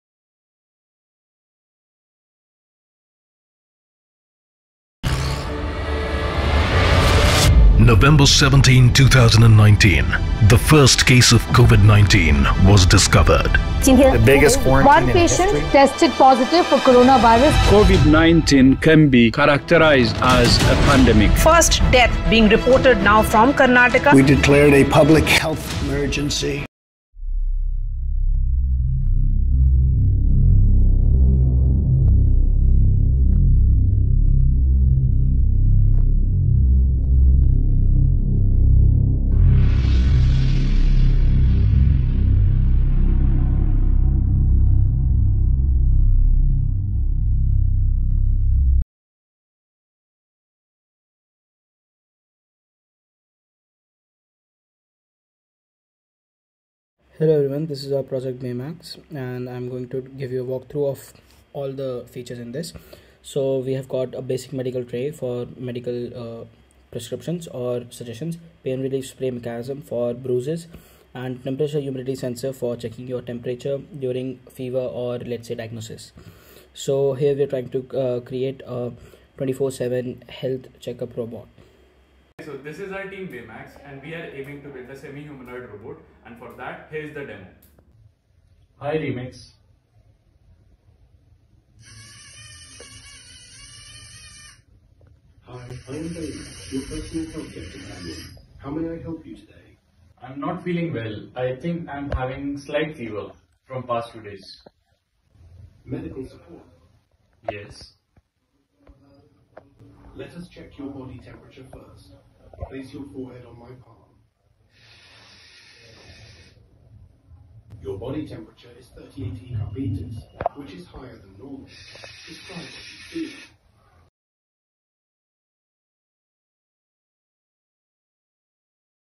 UID - CYIF20061 | Team Name - BayMax | TOP 1000 Finalist | College Youth Ideathon 2025
🔥 Meet the Innovators of Tomorrow! 🔥

🎉 Congratulations, Team BayMax! Your idea has made it to the TOP 1000 in College Youth Ideathon 2025 – where the brightest young minds turn ideas into reality!

📌 UID: CYIF20061
📌 Students: Sneha Agrawal, Meet Jain, Sneha Chaudhary, Siddhartha Khandelwal and Sarvagya Saxena
🏫 College: GLA University Mathura

💡 Big Idea: BAYMAX - The Most Caring Robot Made in India. A revolutionary companion that combines compassion and technology for a healthier tomorrow!

🗣 Tagline: BAYMAX – THE MOST CARING ROBOT MADE IN INDIA!

Join us in celebrating the spirit of innovation and youth-driven change! 💡✨